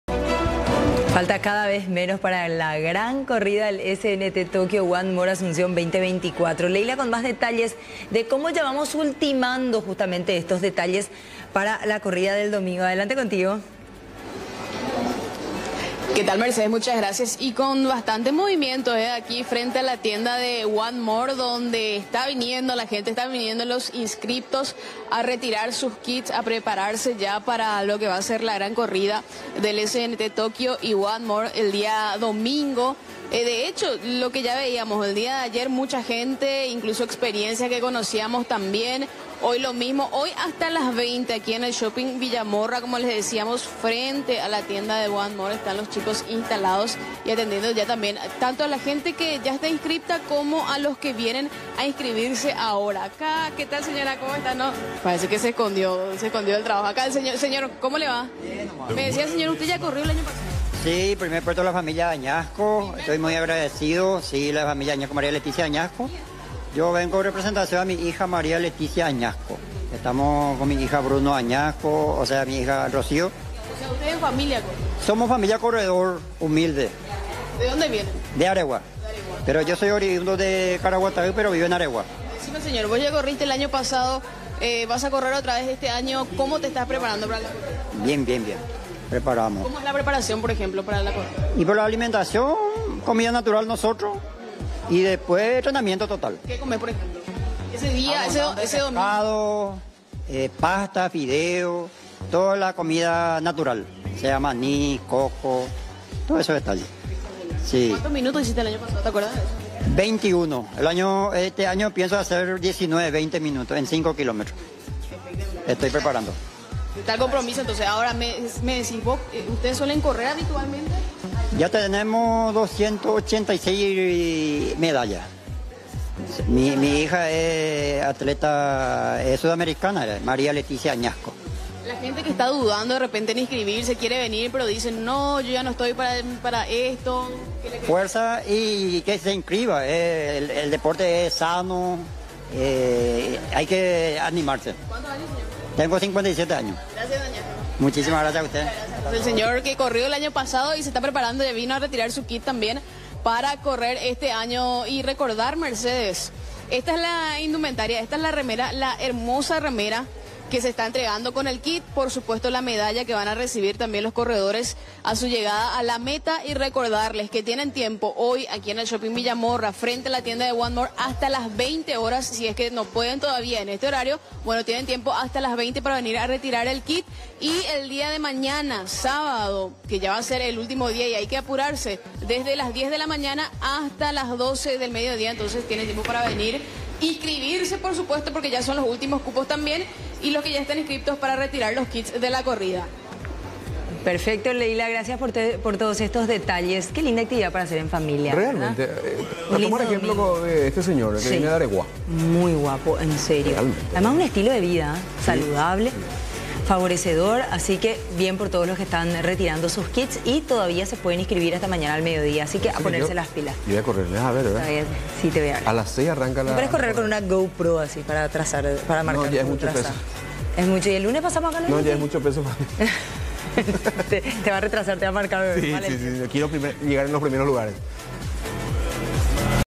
A días de la Corrida Histórica del SNT
Falta cada vez menos para la gran corrida del SNT que se realizará este domingo.

Este domingo vamos a vibrar con la Corrida Histórica del SNT y los participantes continúan retirando sus kits del Shopping Villamorra.

"Yo vengo en representación de mis hijos, somos una familia corredora de Areguá", afirmó un participante.

Y vos, ¿Te lo pensás perder?

Seguí informándote por